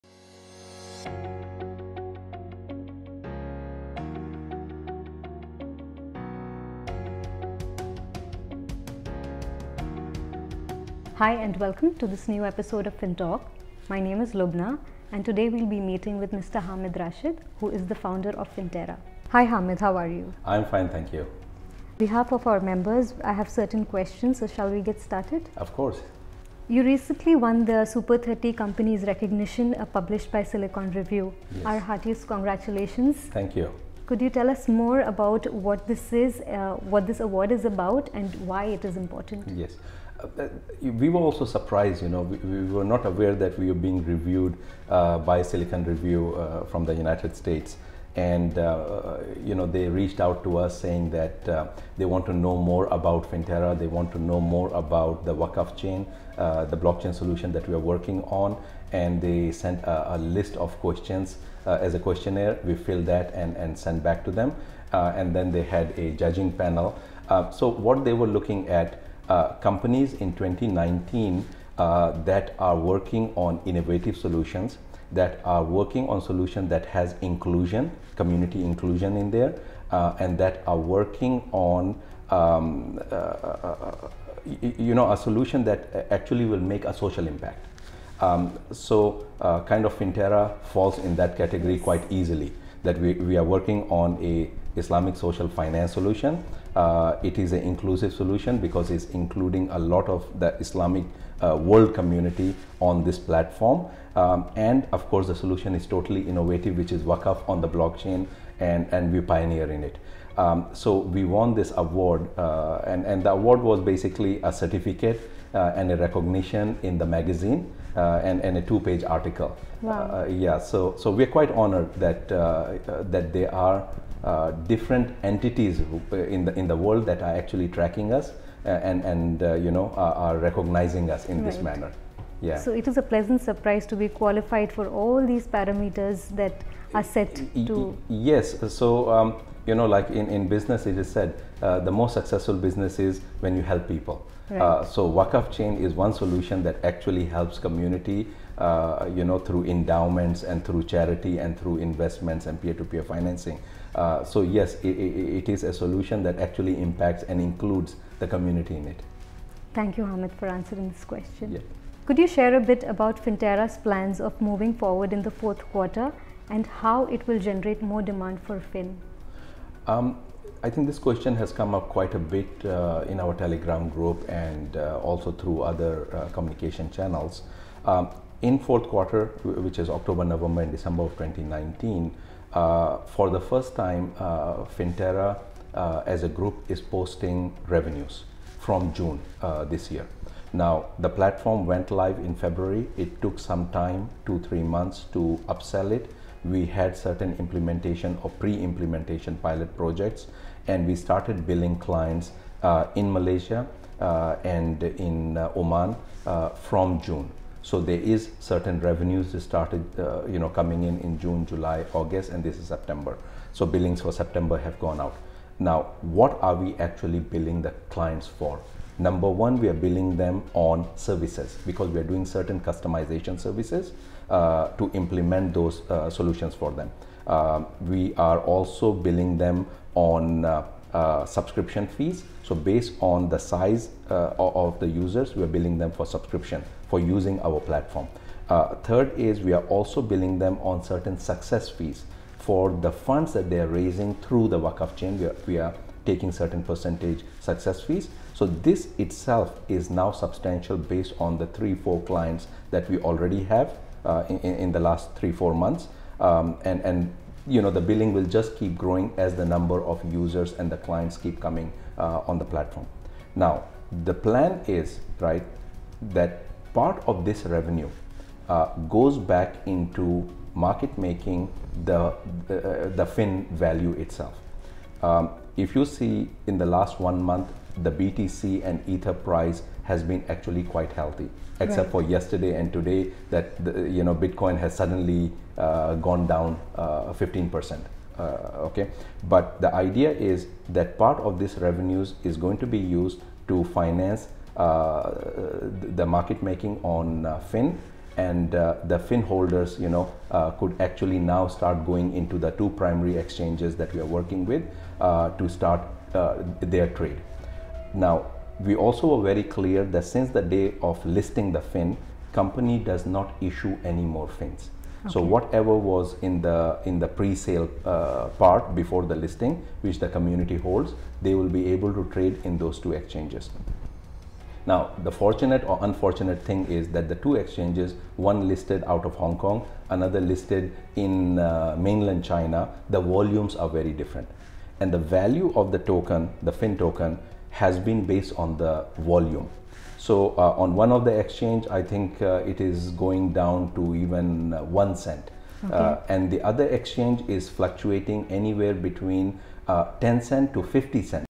FINTALK with Hamid Rashid & Lubna S02E29
In this episode of FINTALK, the Founder of Finterra, Hamid Rashid and Lubna answer questions about the Silicon Review's Super 30 Companies of the Year 2019, Finterra's plans for the rest of the year & more.